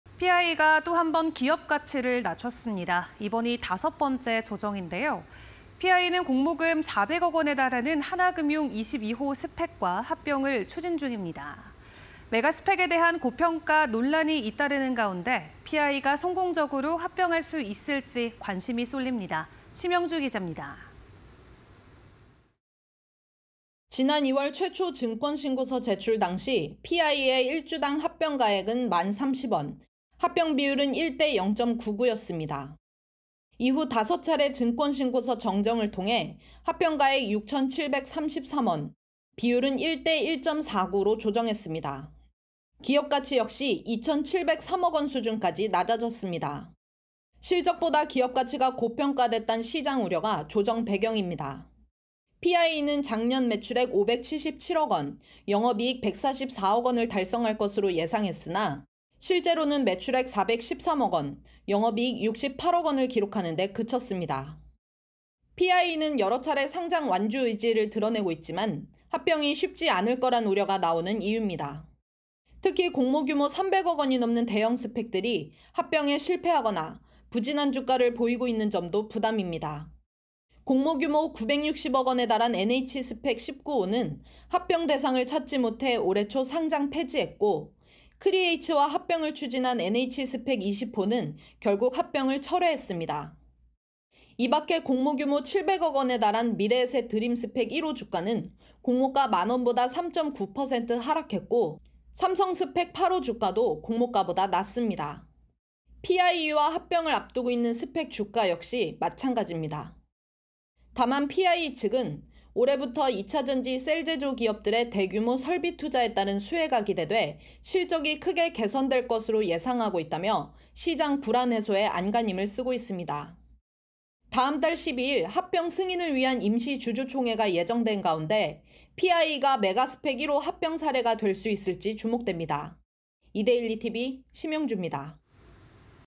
5번째 몸값 낮춘 피아이이…'메가스팩 1호' 가능성은?
[앵커]
피아이이가 또 한번 기업가치를 낮췄습니다. 이번이 다섯 번째 조정인데요. 피아이이는 공모금 400억원에 달하는 하나금융25호스팩과 합병을 추진 중입니다.
메가스팩에 대한 고평가 논란이 계속되는 가운데 피아이이가 성공적으로 합병할 수 있을지 관심이 쏠립니다.
심영주 기자가 보도합니다.